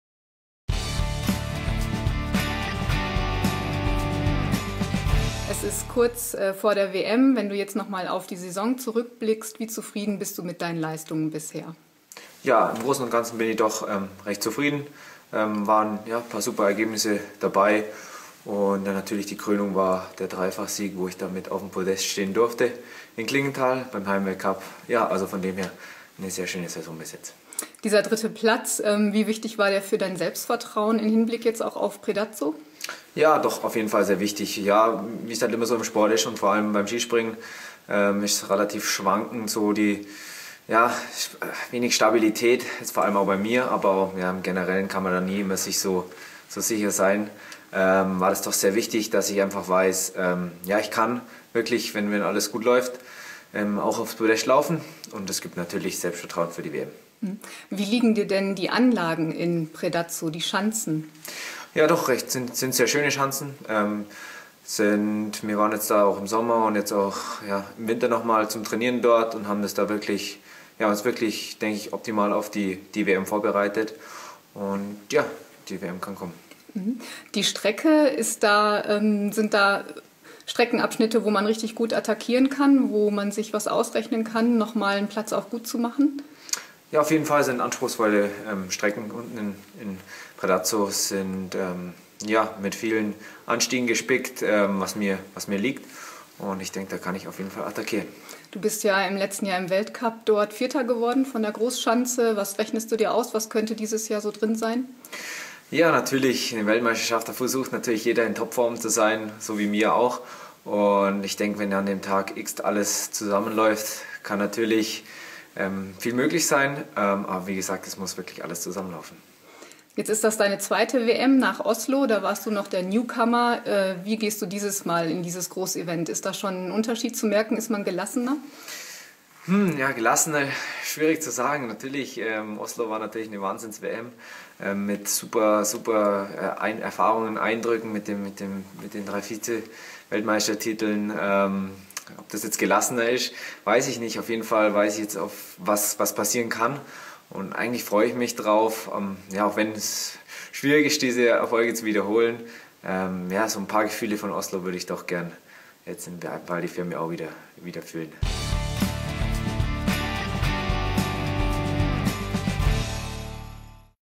Nordische Kombination: Interview mit Johannes Rydzek (12.02.2013)
Johannes Rydzek über die Vorbereitung auf die Nordischen Ski-Weltmeisterschaften 2013 in Val di Fiemme (ITA).